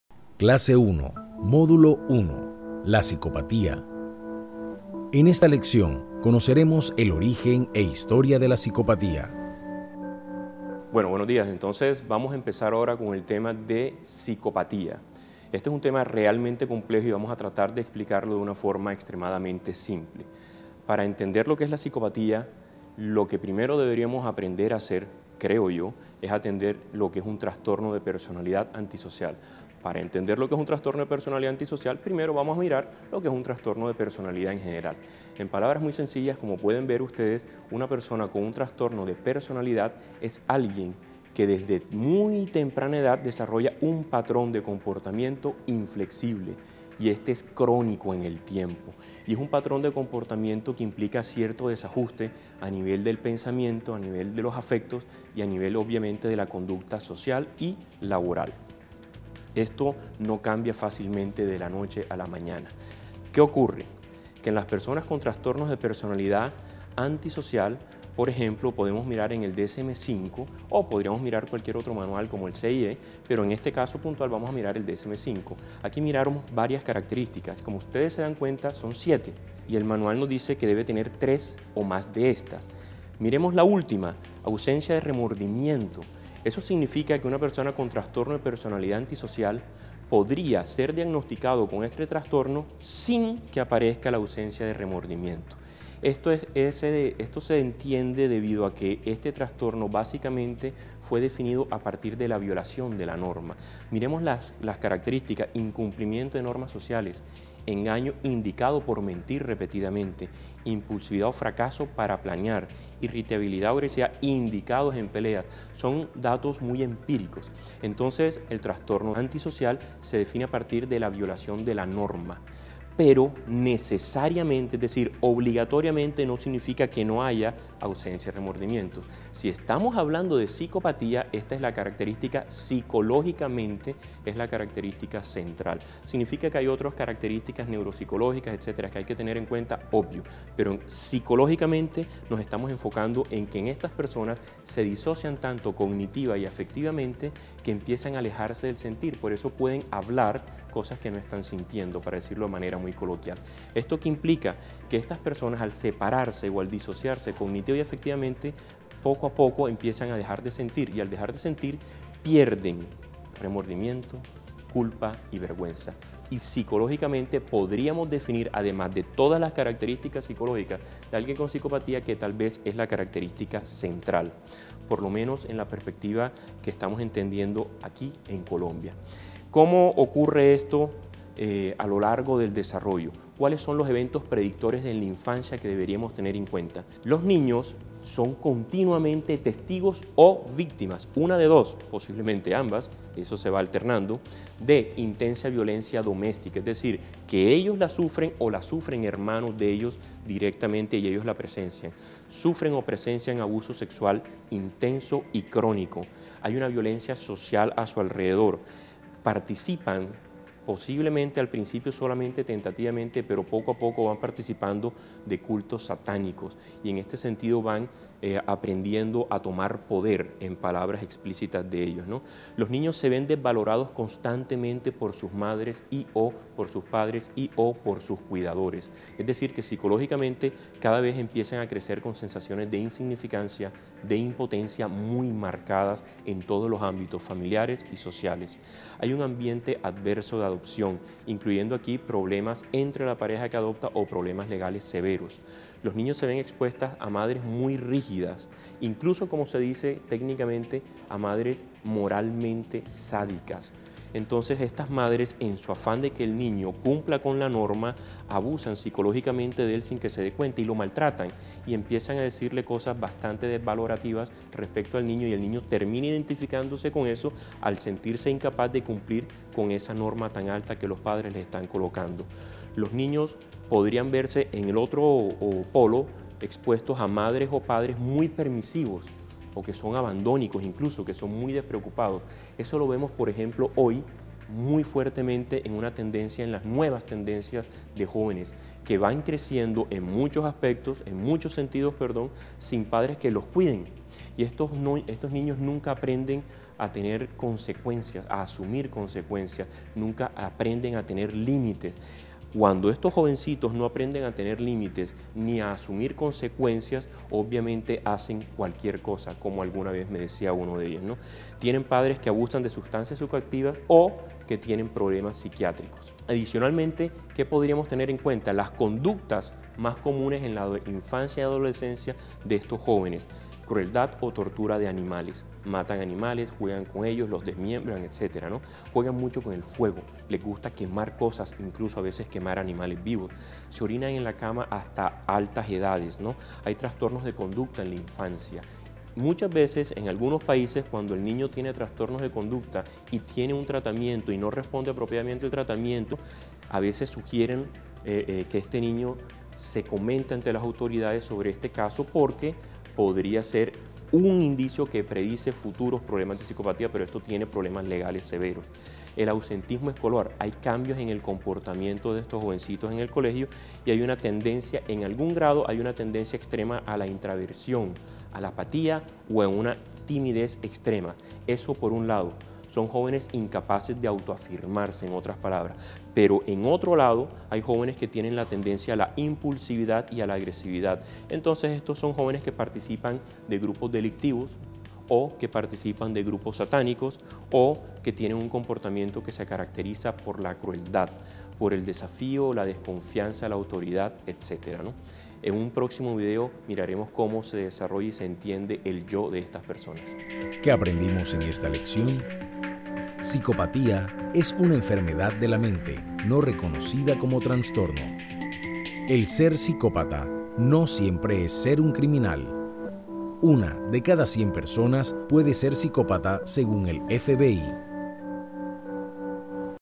Psicopatía criminal - Módulo 2, lección 2
Lección 2, módulo 2, del curso gratuito  "Introducción a la psicopatía criminal" ,  MOOC de la Universidad del Norte.